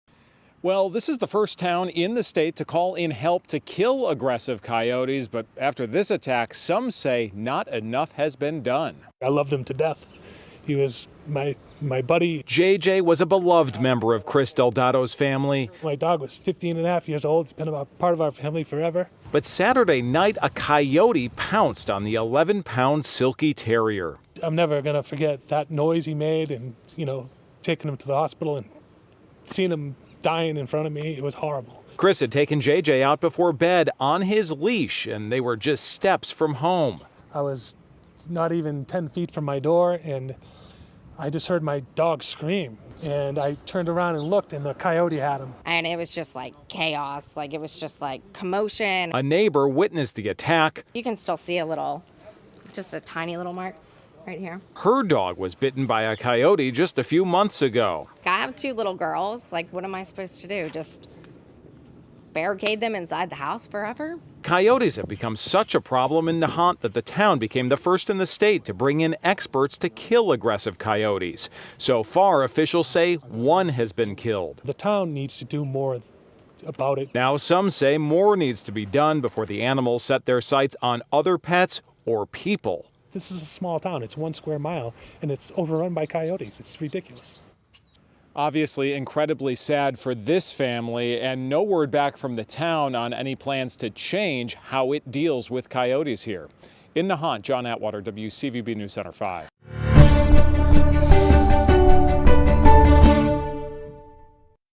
'Never going to forget the noise': Dog killed by coyote close to its Mass. home
Nahant was the first town in Massachusetts to call for federal help to kill aggressive coyotes, but some say not enough has been done.